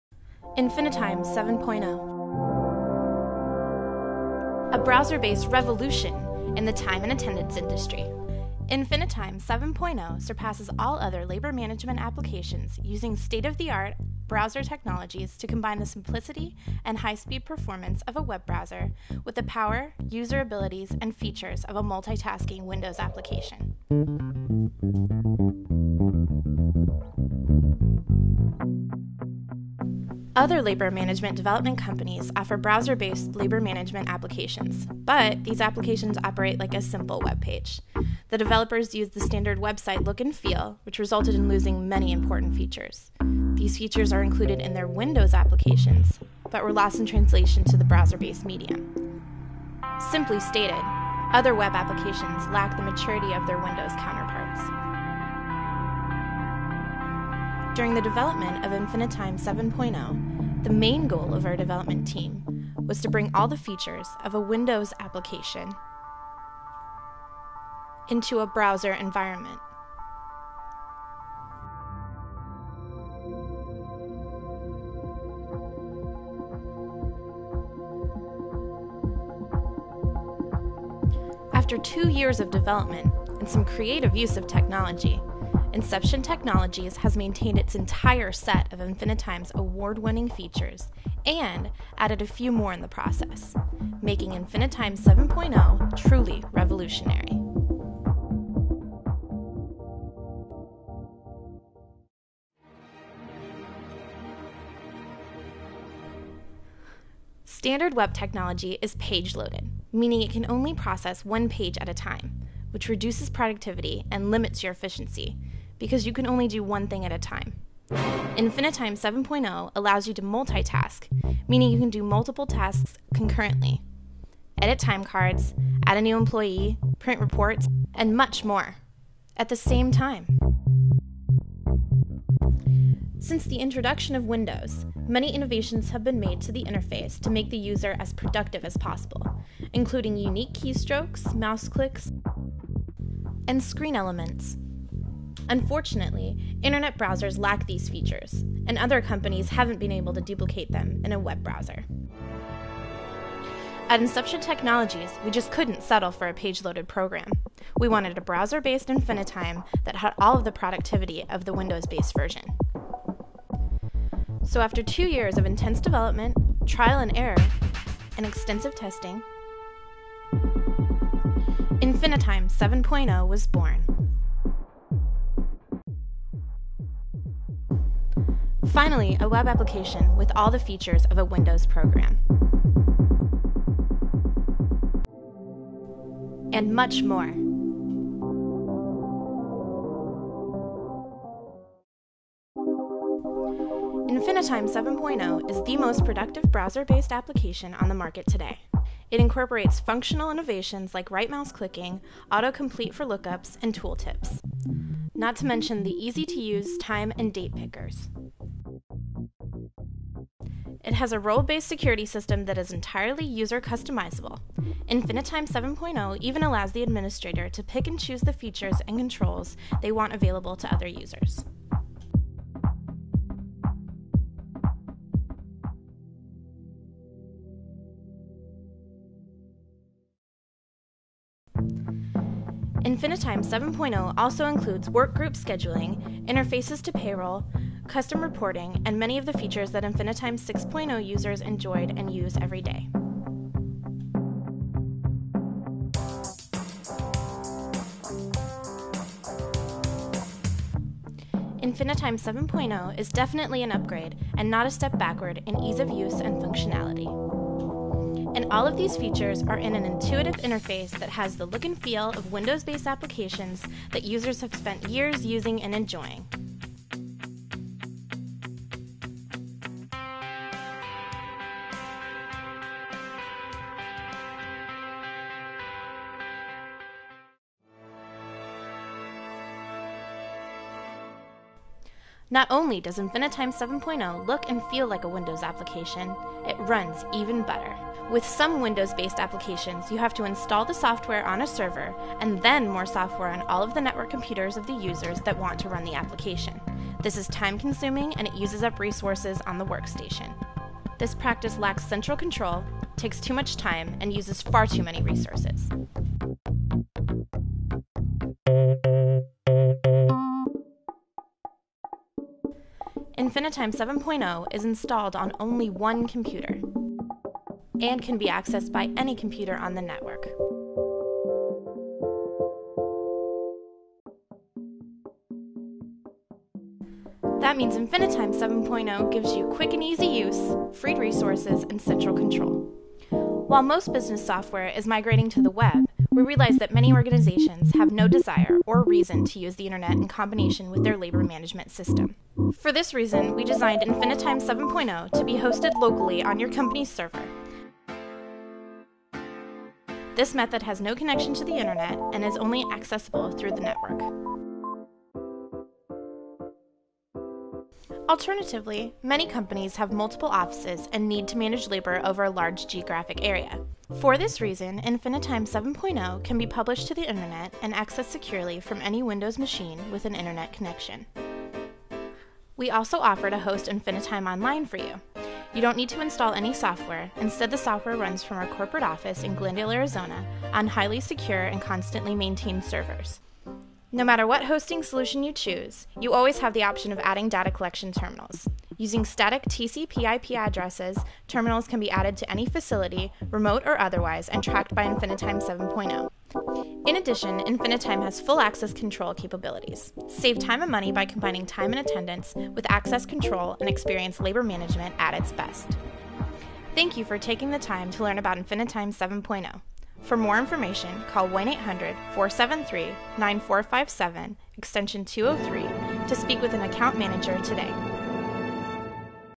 InfiniTime Overview Video for Web
Inception Technologies', InfiniTime™ 7.0 Labor Management Software is a true client/server solution based on an Oracle database architecture and ASP.net. InfiniTime™ utilizes a relational database on an Oracle database server. All the software modules in InfiniTime™ share the same database and are integrated in this SQL environment.